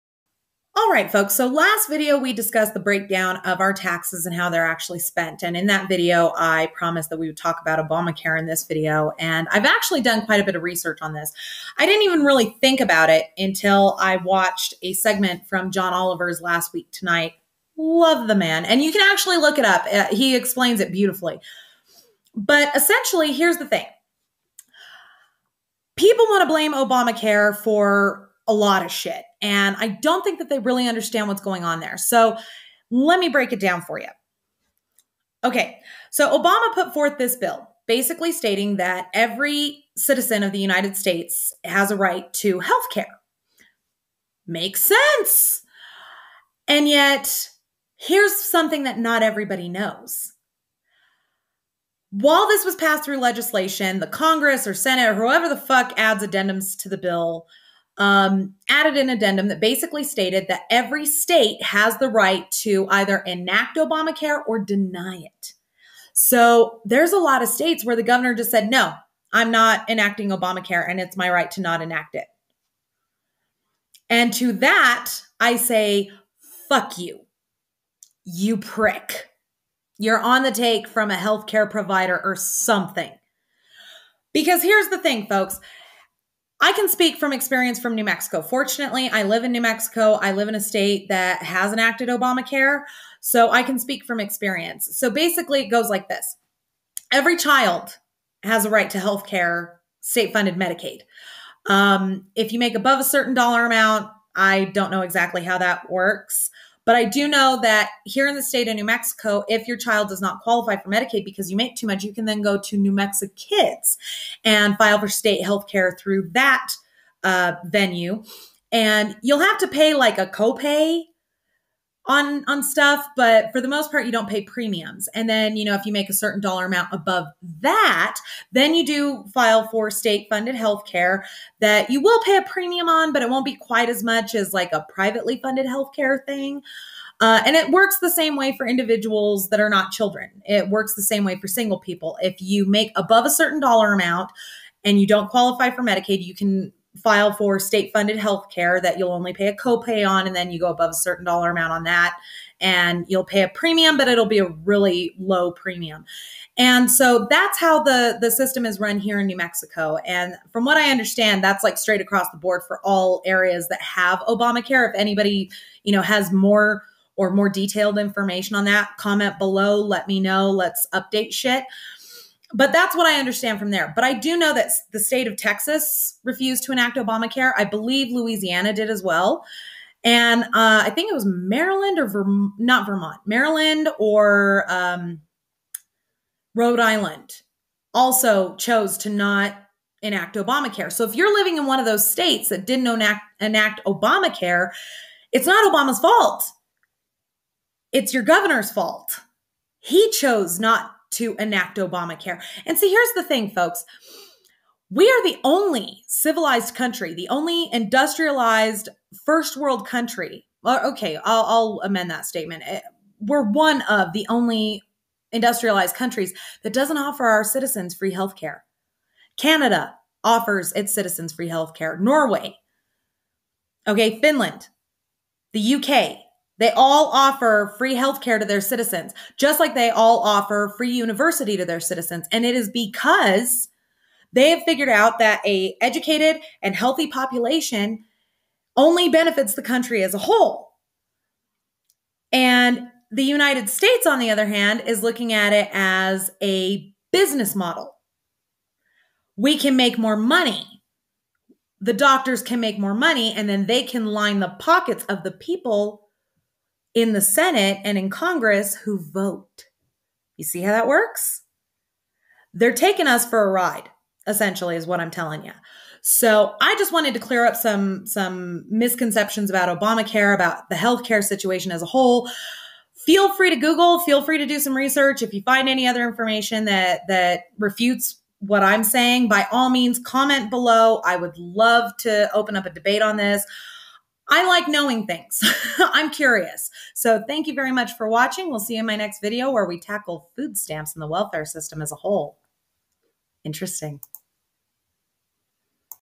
ObamaCare is NOT the problem!!
Talk to your Governor. They are the ones keeping you sick. Gotta love those medical company kickbacks!